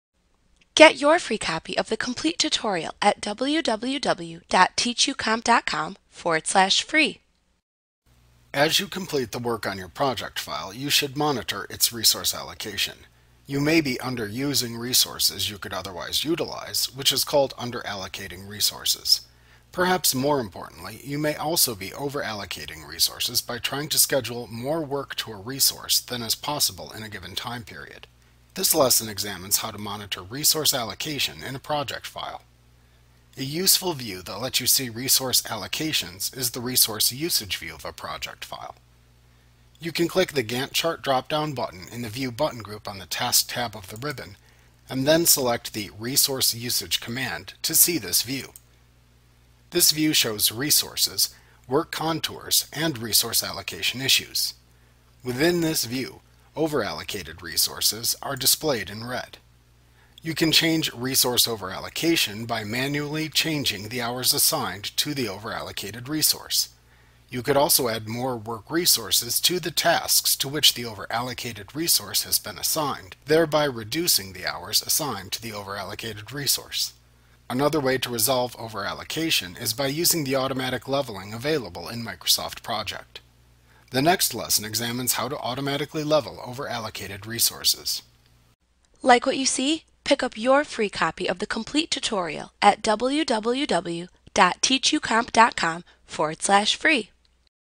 Project 2016 Tutorial Monitoring Resource Allocation Microsoft Training
Learn about Monitoring Resource Allocation in Microsoft Project A clip from Mastering Project Made Easy v. 2016. - the most comprehensive Project tutorial available. Visit us today!